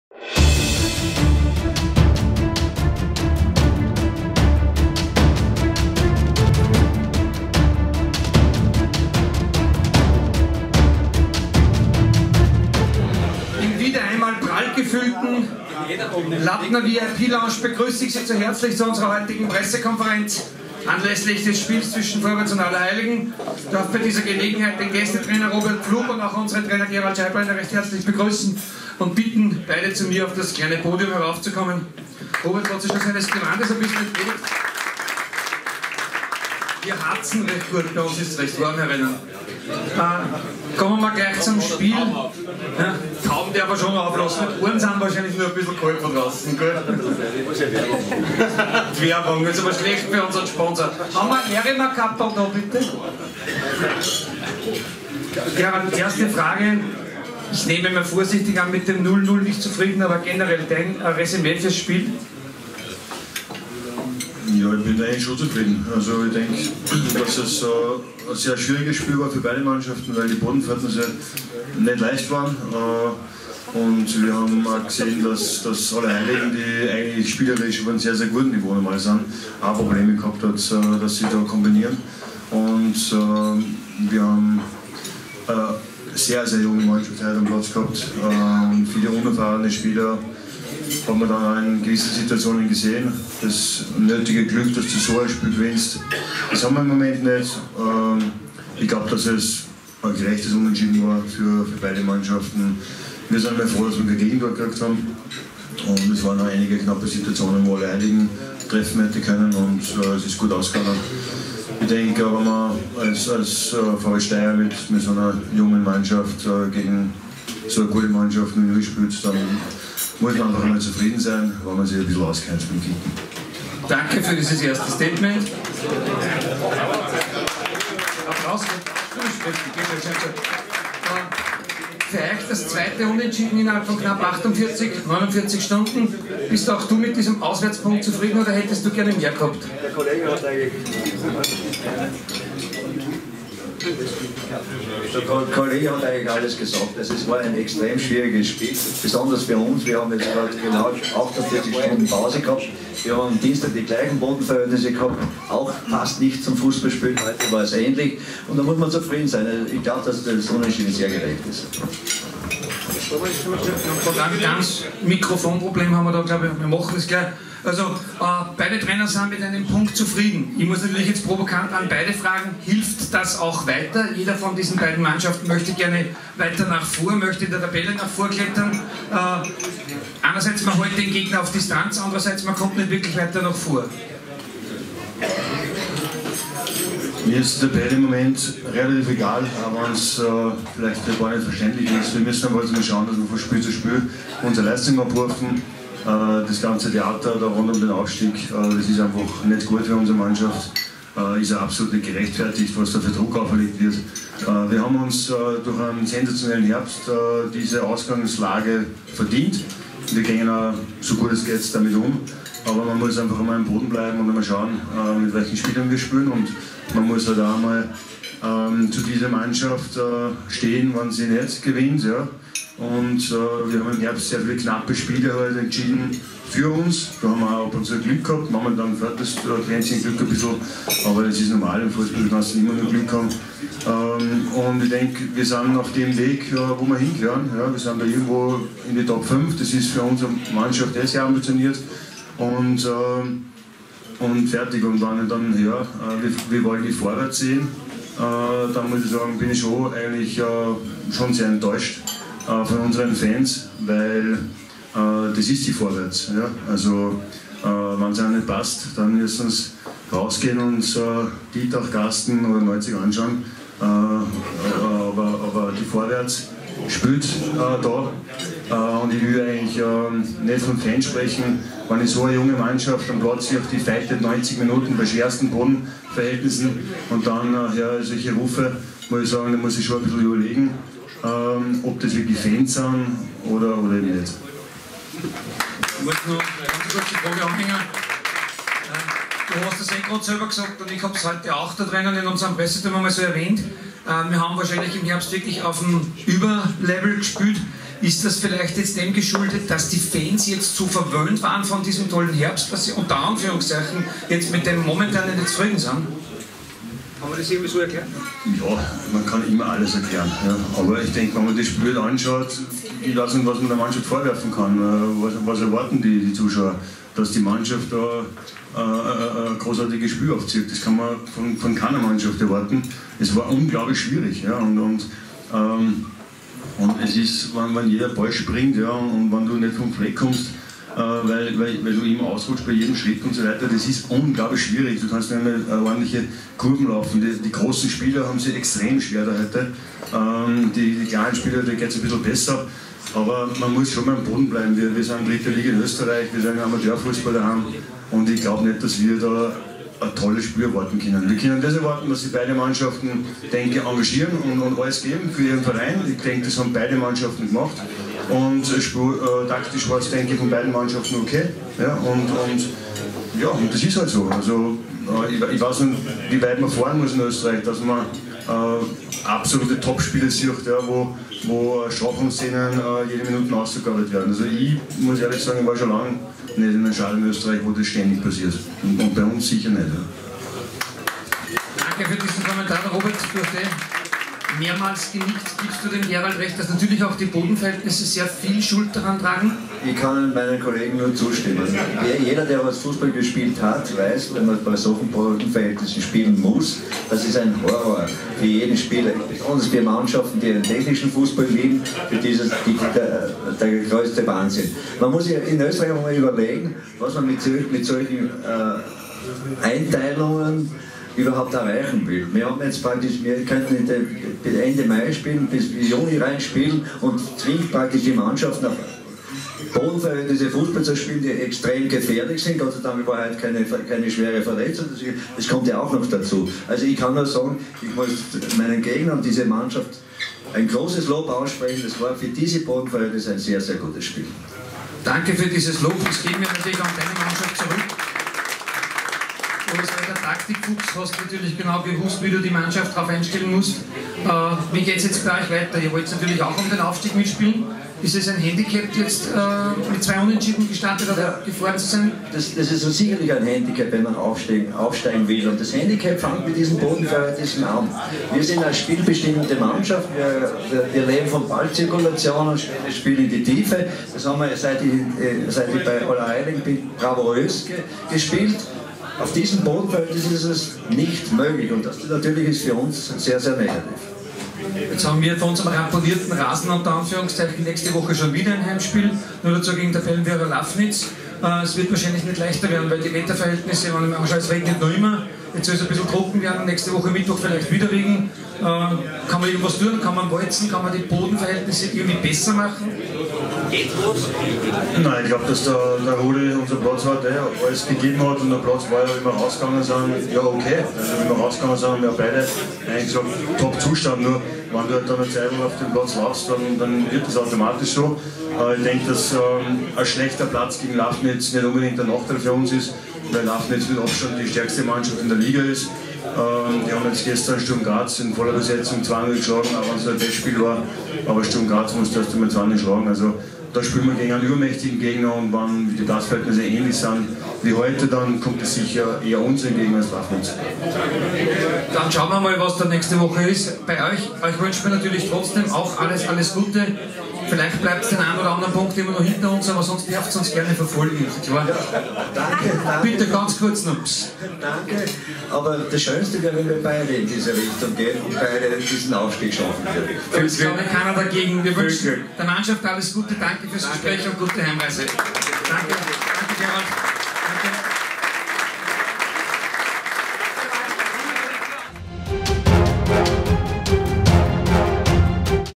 Pressekonferenz SK Vorwärts Steyr vs. USV Allerheiligen
Bei sehr schwierigen Bodenverhältnissen trennen sich Vorwärts Steyr und Allerheiligen mit einem leistungsgerechten 0:0 Unentschieden. Nach dem Spiel stehen die beiden Trainer Gerald Scheiblehner und Robert Pflug den anwesenden Journalisten und VIPs bei der obligatorischen Pressekonferenz Rede und Antwort.

Nächstes Heimspiel: 6. April 2018 | 18:55 Uhr | SK Vorwärts Steyr vs. SV Lafnitz | LIFE RADIO Arena Steyr

SK Vorwärts Steyr - Tradition seit 1919